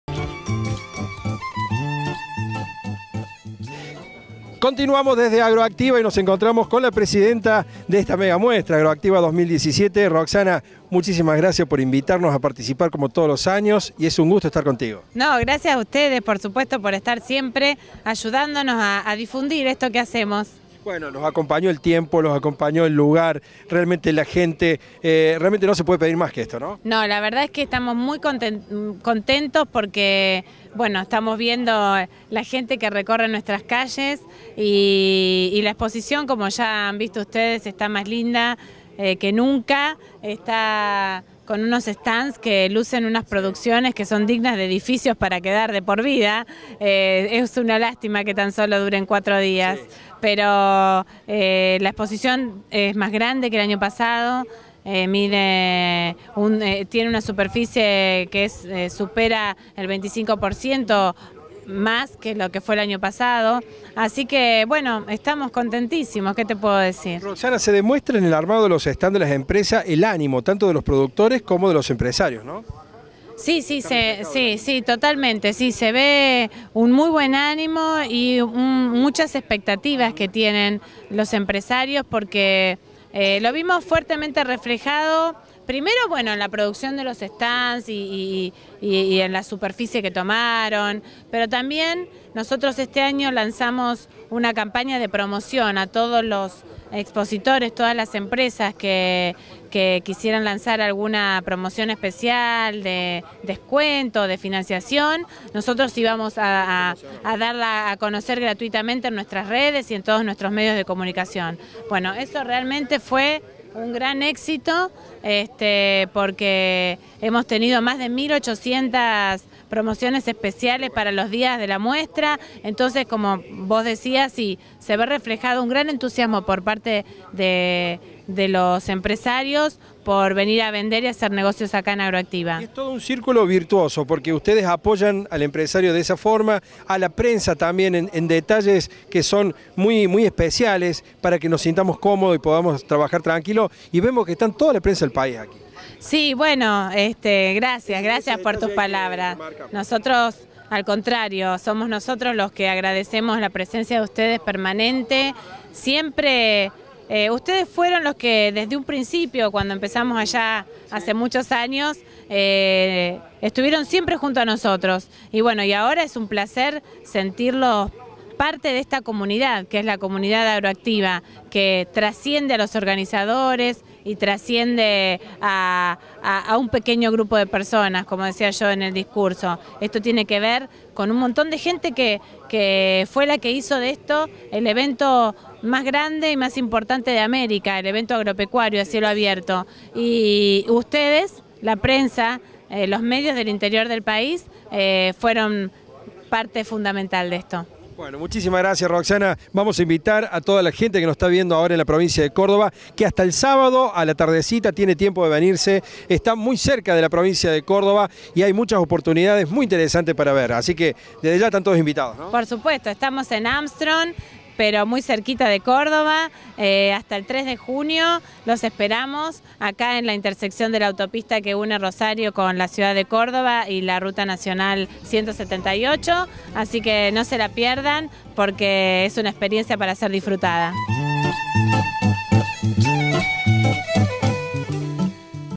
Agroactiva 2017 - Rosana Nardi, presidente de Agroactiva.
Un panorama de lo que seguramente es la Exposición más grande de Argentina, hablamos con Rosana Nardi, presidente de Agroactiva.